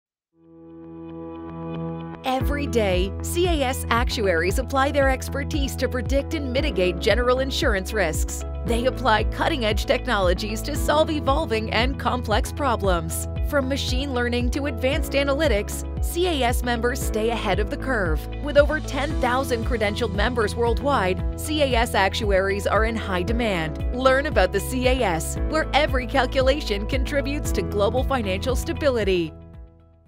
A Global Society of General Insurance Actuaries
General Insurance actuaries credentialed by the CAS are leading efforts to mitigate risks and solve real-world problems, using advanced analytics and cutting-edge technology to create safer, more resilient communities.